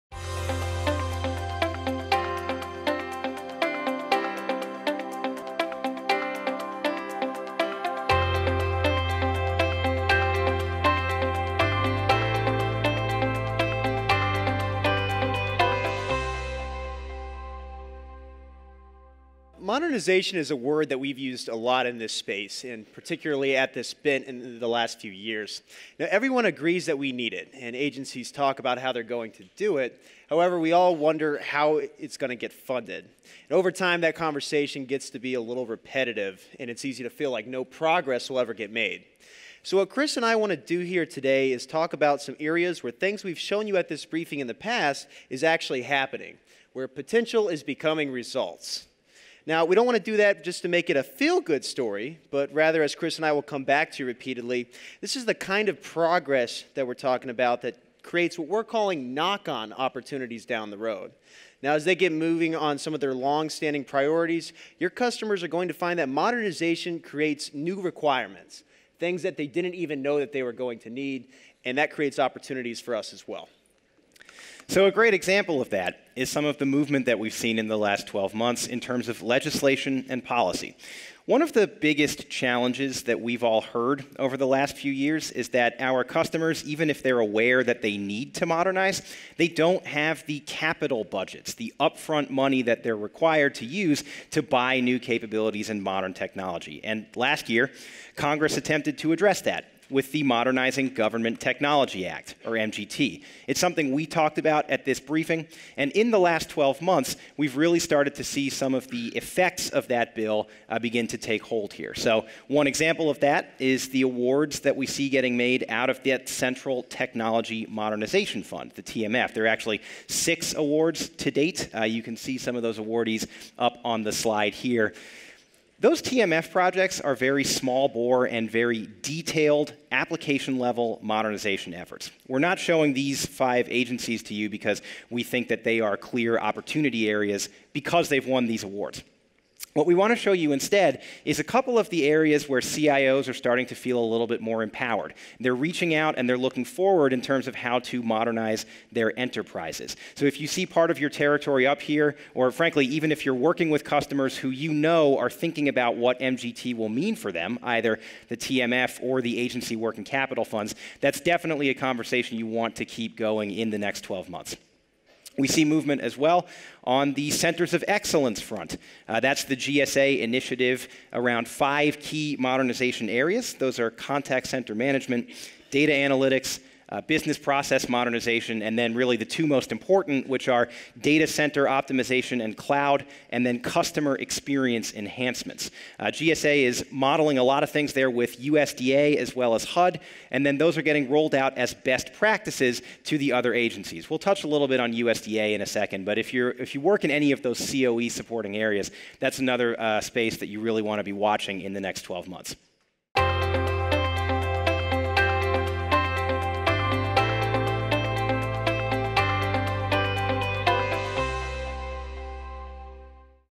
FY19 Federal Civilian IT Budget Snapshot -- Market analysts give IT Modernization update
Market intelligence analysts talk about the progress federal civilian agencies have made with the modernization initiative and new opportunities being created.